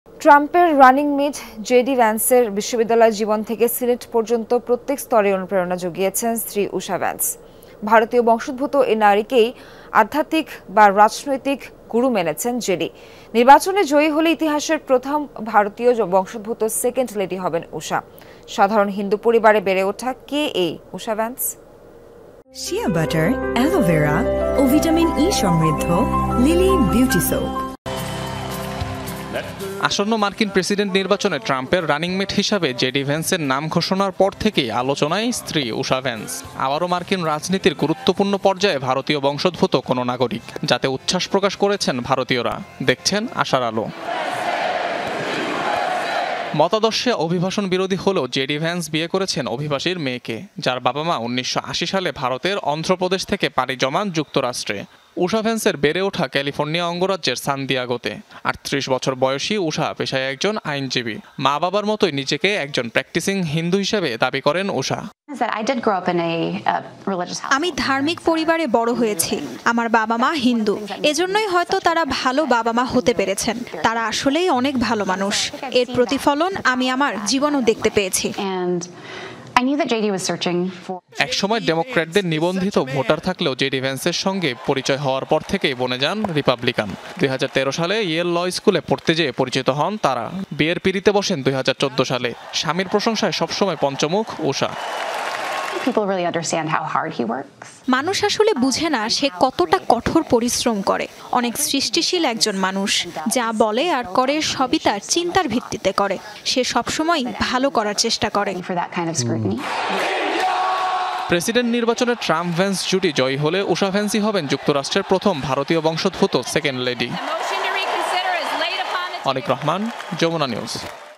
ট্রাম্পের রানিং মেট ভ্যান্সের স্ত্রী কে এই ঊষা ভ্যান্স? | Wife of Trumps Running mate | Jamuna TV
ট্রাম্পের রানিংমেট জেডি ভ্যান্সের বিশ্ববিদ্যালয় জীবন থেকে সিনেট পর্যন্ত প্রত্যেক স্তরেই অনুপ্রেরণা জুগিয়েছেন, স্ত্রী ঊষা ভ্যান্স। ভারতীয় বংশোদ্ভুত এই নারীকেই আধ্যাত্মিক বা রাজনৈতিক গুরু মেনেছেন, জেডি। নির্বাচনে জয়ী হলে, ইতিহাসের প্রথম ভারতীয় বংশোদ্ভূত সেকেন্ড লেডি হবেন ঊষা। সাধারণ হিন্দু পরিবারে বেড়ে ওঠা...কে এই ঊষা ভ্যান্স?

Address:
Bangladesh.